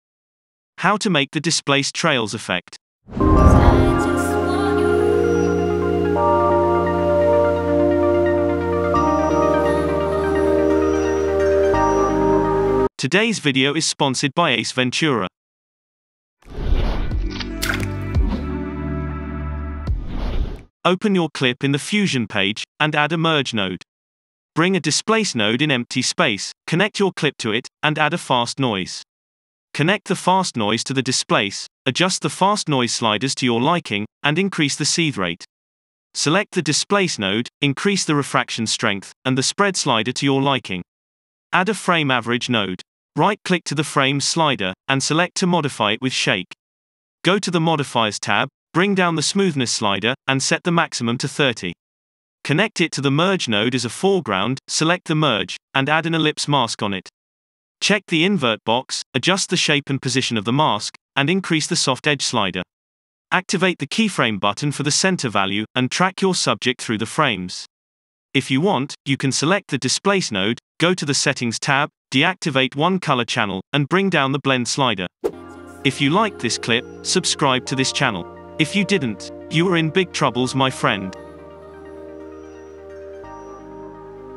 Displaced Trails Effect | DaVinci Resolve 18
How to make the displaced trails effect in davinci resolve fusion page

you can send them here :

BTC : 1Ecg6F8UnFHnVxsKhbkecqtge11vBhqS5
ETH : 0xB643C1C8B6C75bcB3426F40f8514CB89Fc294abC